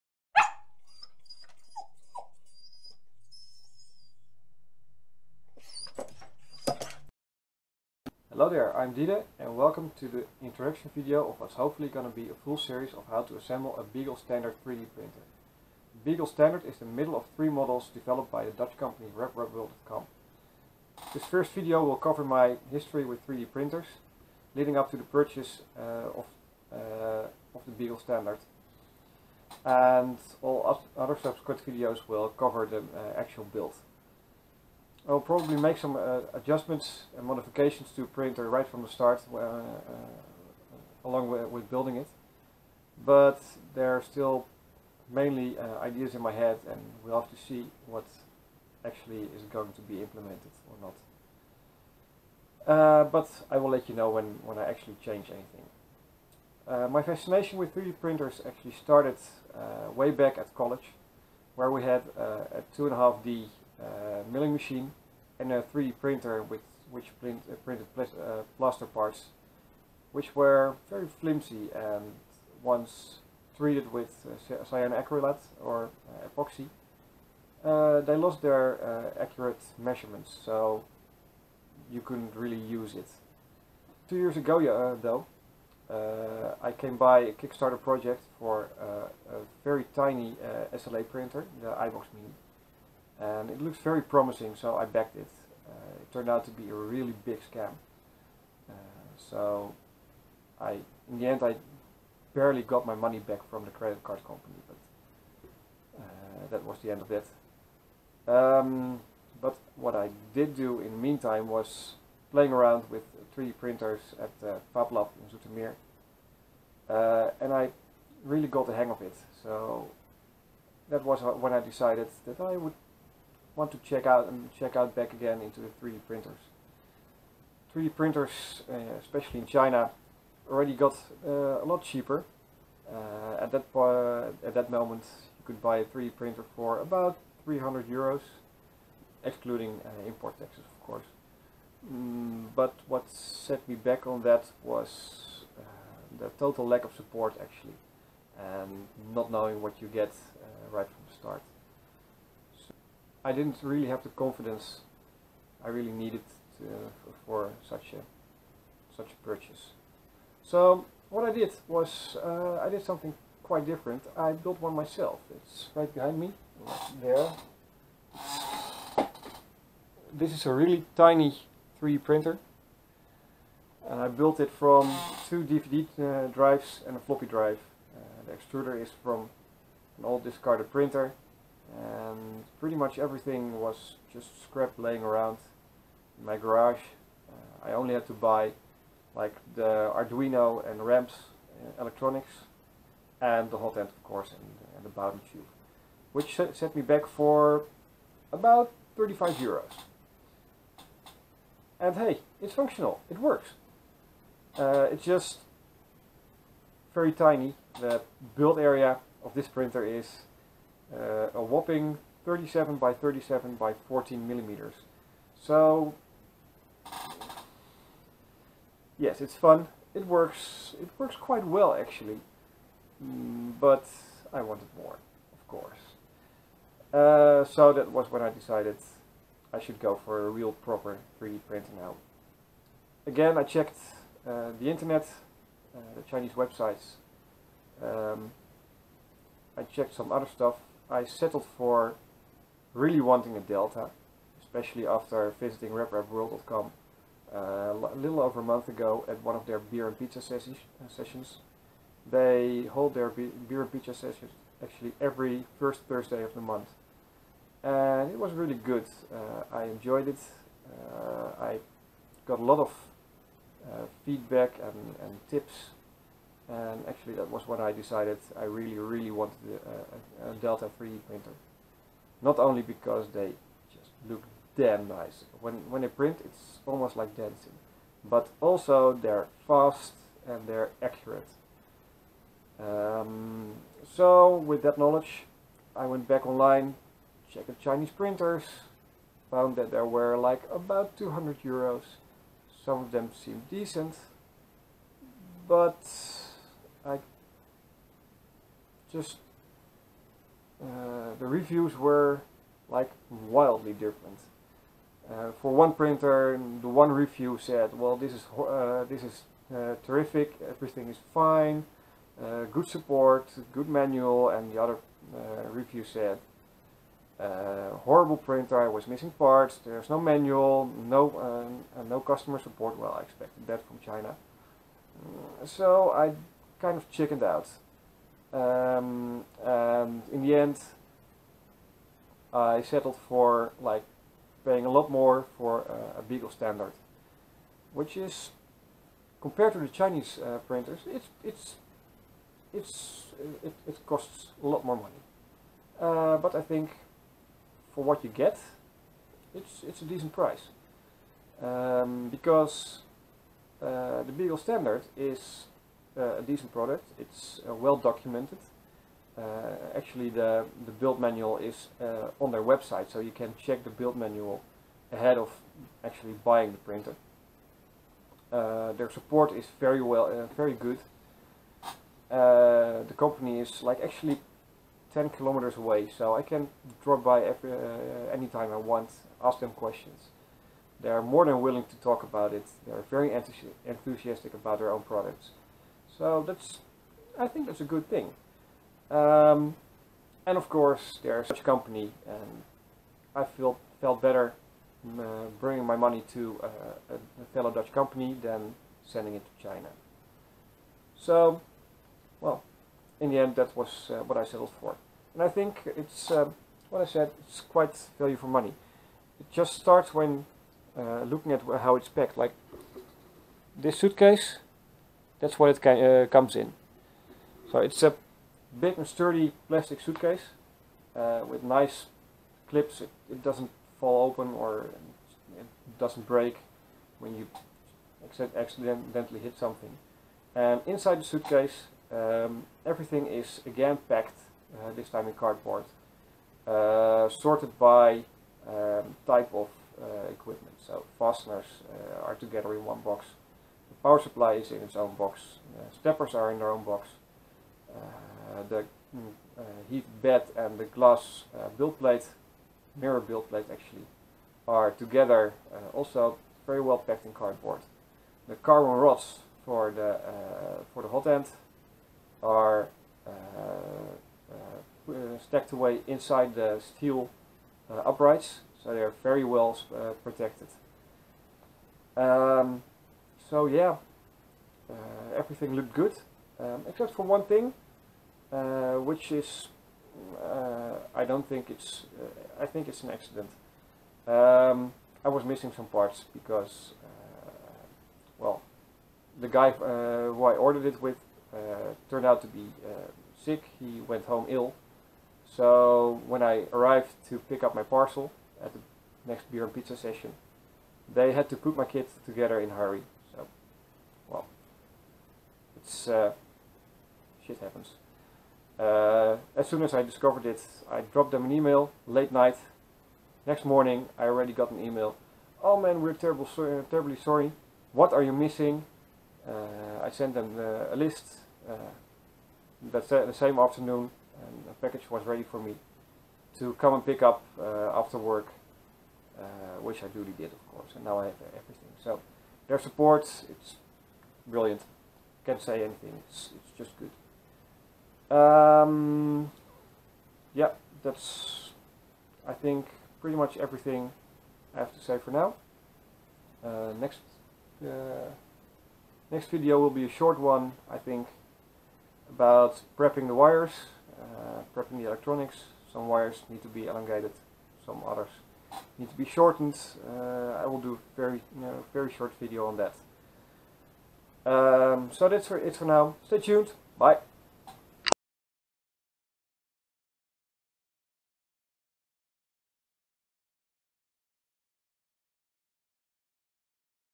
Beagle Build Log Episode 1: Introduction
This video is an introduction to what is to become a full series of Build Log videos for the assembly of my Reprapworld.com Beagle Standard 3D Delta printer. Full instructions can be found on their website and I will be largely following their instructions throughout the build - although I am thinking of some modifications but that will need to take shape as I go along.

This video will largely cover the process I went through leading up to the purchase of this 3D-printer kit, as well as a quick rundown of the kit itself.

I will try to upload videos following the chapter buildup from the manual, so you can follow my progress and read up ahead if you like so.